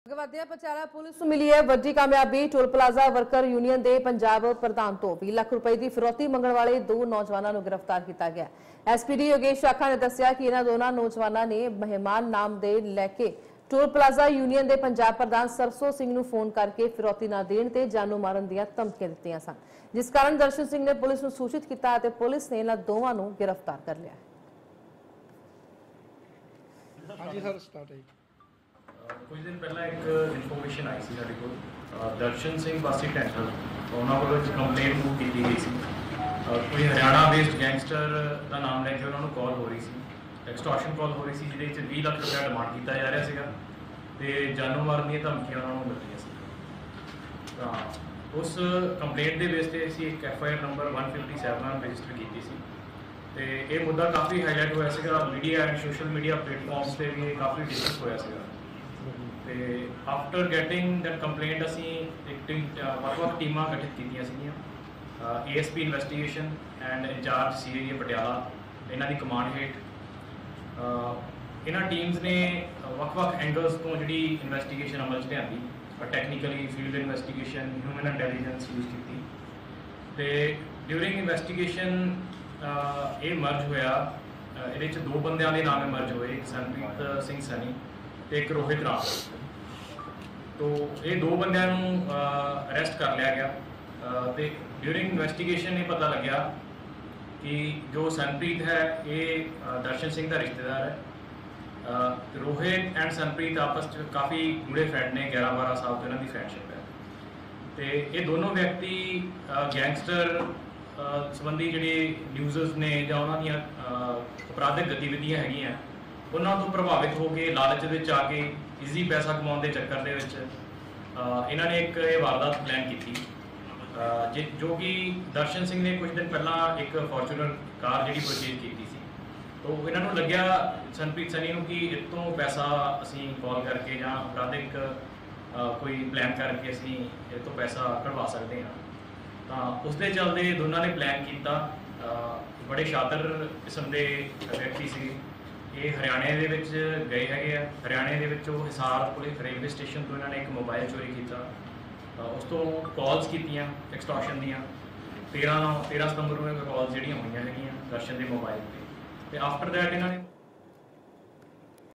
Patiala News | Punjab Police | 20 ਲੱਖ ਦੀ ਫਿਰੌਤੀ ਮੰਗਣ ਵਾਲੇ 2 ਨੌਜਵਾਨ ਗ੍ਰਿਫਤਾਰ | India News Punjab
ਪਟਿਆਲਾ ਪੁਲਿਸ ਨੂੰ ਮਿਲੀ ਵੱਡੀ ਕਾਮਯਾਬੀ, ਟੋਲ ਪਲਾਜ਼ਾ ਵਰਕਰ ਯੂਨੀਅਨ ਦੇ ਪੰਜਾਬ ਪ੍ਰਧਾਨ ਤੋਂ 20 ਲੱਖ ਦੀ ਫਿਰੌਤੀ ਮੰਗਣ ਵਾਲੇ ਦੋ ਨੌਜਵਾਨਾਂ ਨੂੰ ਕੀਤਾ ਗ੍ਰਿਫਤਾਰ. SPD ਯੋਗੇਸ਼ ਸ਼ਾਖਾ ਨੇ ਦੱਸਿਆ ਕਿ ਇਨ੍ਹਾਂ ਦੋਵਾਂ ਨੌਜਵਾਨਾਂ ਨੇ ਮਹਿਮਾਨ ਦਾ ਨਾਂ ਲੈ ਕੇ ਟੋਲ ਪਲਾਜ਼ਾ ਯੂਨੀਅਨ ਦੇ ਪੰਜਾਬ ਪ੍ਰਧਾਨ ਸਰਸੋ ਸਿੰਘ ਨੂੰ ਫੋਨ ਕਰ ਕੇ ਫਿਰੌਤੀ ਨਾ ਦੇਣ ’ਤੇ ਜਾਨੋਂ ਮਾਰਨ ਦੀਆਂ ਧਮਕੀਆਂ ਦਿੱਤੀਆਂ, ਜਿਸ ਕਾਰਨ ਦਰਸ਼ਨ ਸਿੰਘ ਨੇ ਪੁਲੀਸ ਨੂੰ ਸੂਚਿਤ ਕੀਤਾ ਤੇ ਪੁਲੀਸ ਨੇ ਦੋਵਾਂ ਨੂੰ ਗ੍ਰਿਫ਼ਤਾਰ ਕਰ ਲਿਆ।

Patiala Police got a big success, arrested two youths who demanded a ransom of 20 lakhs from the Punjab President of Toll Plaza Workers Union. SPD Yogesh Branch said that these two youths called Sarso Singh, Punjab President of Toll Plaza Union, taking the name of the guest and threatening to kill him if he did not pay the ransom, due to which Darshan Singh informed the police and the police Both were arrested.